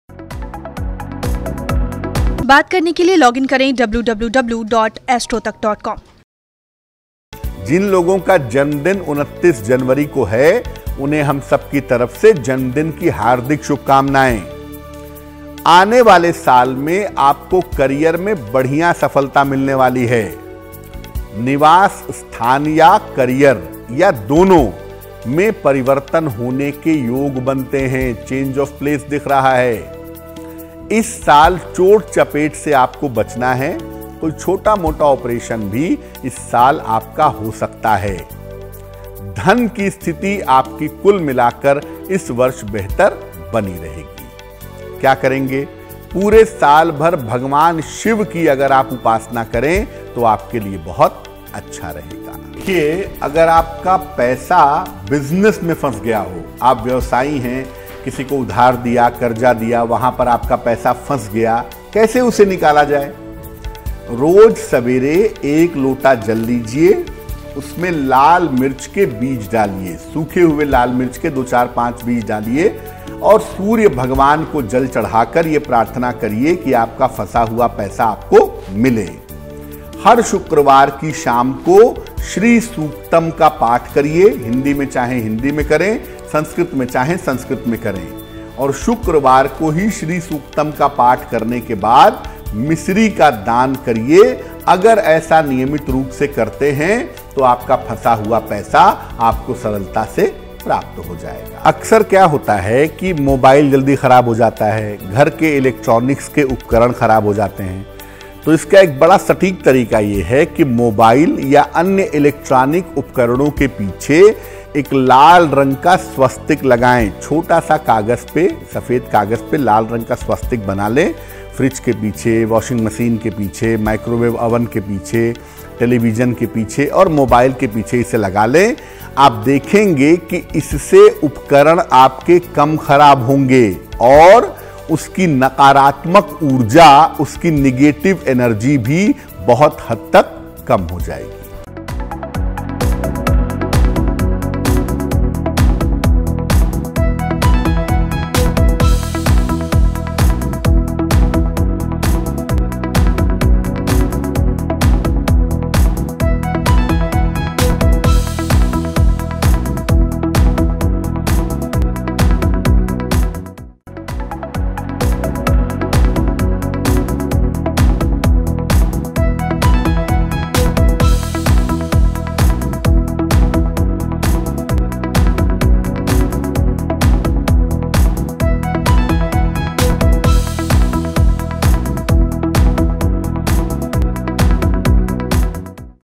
जिनका जन्मदिन 29 जनवरी को है उनका आने वाला एक साल कैसा रहेगा। Shailendra Pandey| Astro Tak
Birthday Special: ज्योतिर्विद शैलेंद्र पांडेय जी से जानते हैं कि जिनका जन्म 29 जनवरी को हुआ है उनका आने वाला एक साल कैसा रहने वाला है.